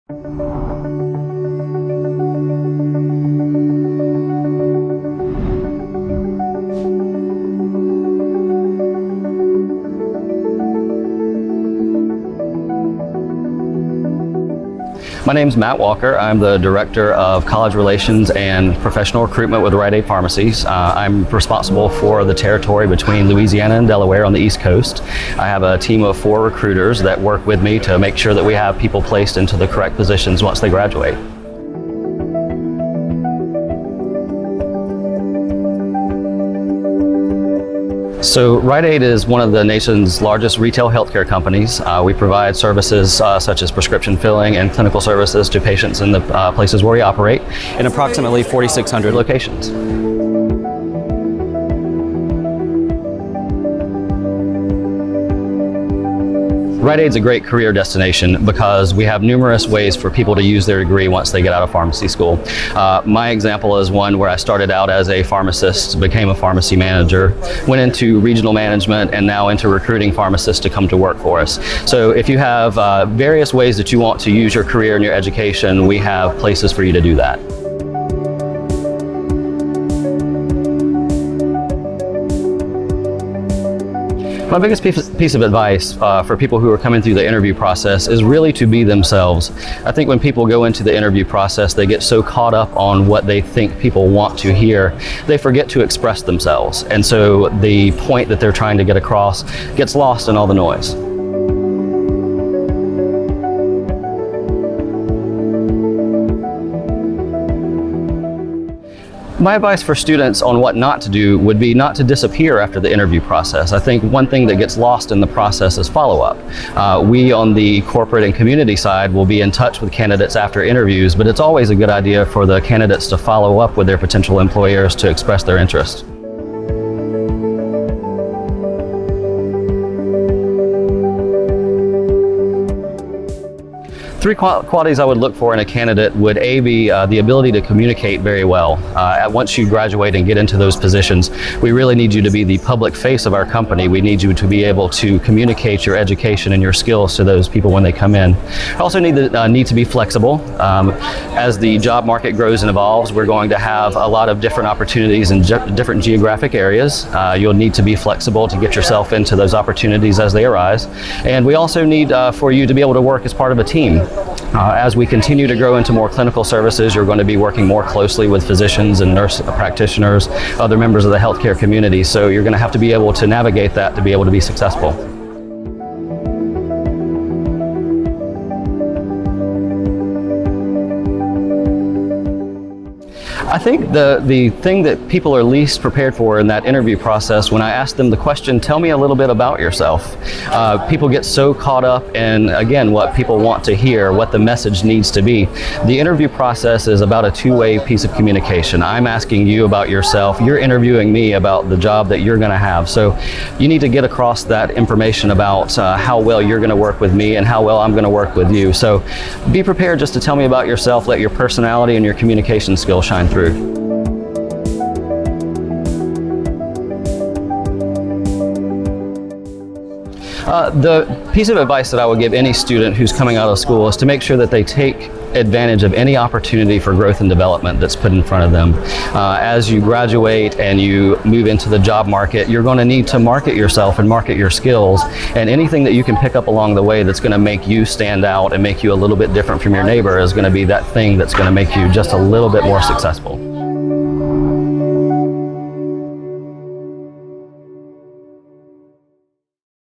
Application and Interviewing Advice for New Graduates | Matt Walker, PharmD | Rite Aid
Join Matt Walker, PharmD, Director College Relations & Professional Recruitment at Rite Aid, as he discusses application and interviewing advice for new pharmacy graduates.

Learn more about RXinsider and our Thought Leader Video Series offering